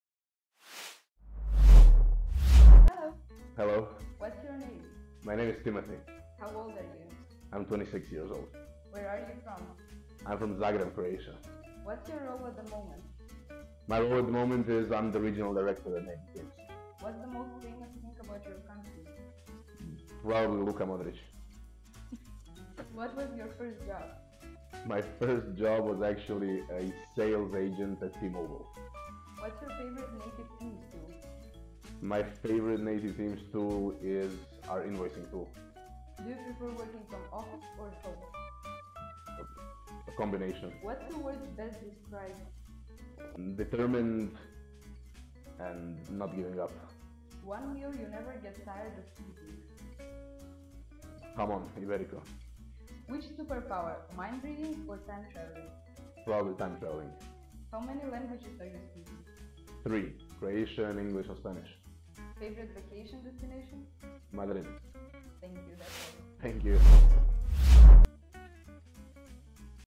60 seconds with Timothei, our Regional Director!
Season 1, Episode 3: 60 seconds with Timothei, our Regional Director of Native Teams!🎥

In this episode, meet Timothei Jukic, the driving force behind our regional success!🗺️
Timi, as we like to call him, comes from the charming streets of Zagreb, Croatia🇭🇷, a place known for its rich culture and historic landmarks.

Curious about Timothei’s career journey, his favourite vacation destination, and how he describes himself?

P.S. Don’t forget to try some Jamón ibérico, it’s Timi’s all-time favourite! 🍖